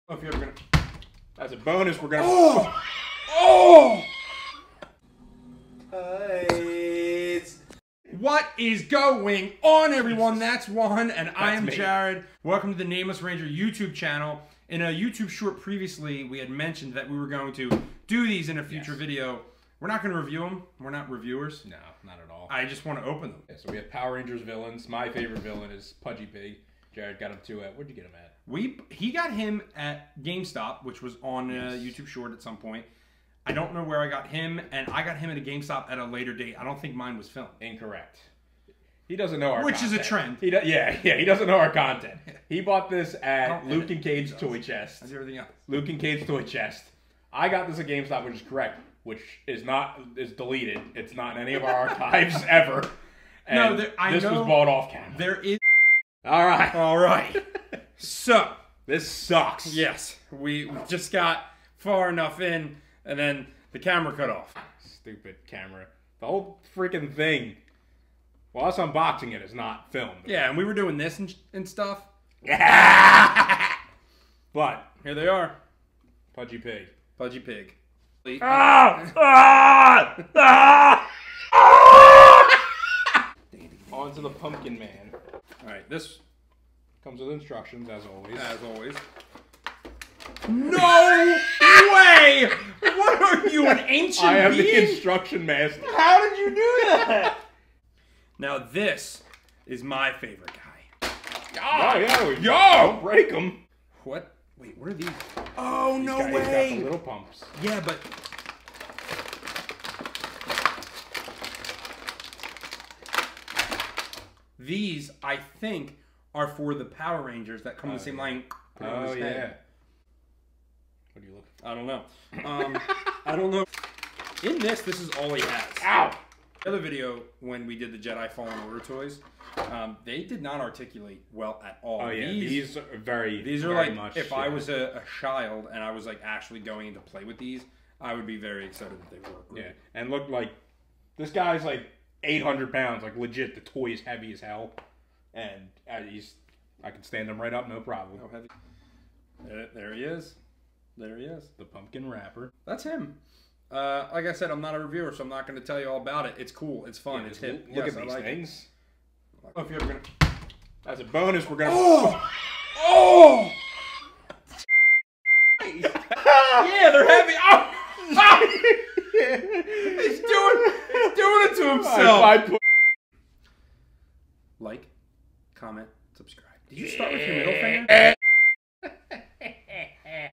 Power Rangers Villians Unboxing!
Unboxing some devious Power Rangers villians!